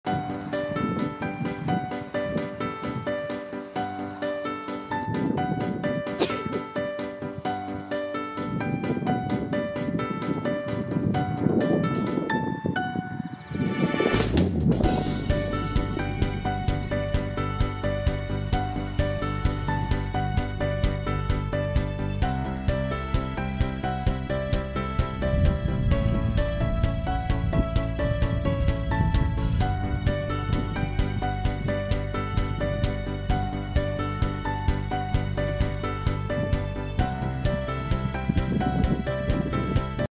Autumn Nasu Flower World Tourism [plumed cockscomb] 那須フラワーワールド ケイトウ
"Nasu Flower World" in Nasu Town, Tochigi Prefecture, where beautiful flower fields spread throughout the four seasons with the Nasu mountain range in the background.
In autumn, it is colored with 3 million bright flowers called "Celosia".
From the site at an altitude of about 600m, the location overlooking the Nasu Highlands is also wonderful, making it the most famous spot in the Nasu Highlands.

*Shooting date
2022/09/17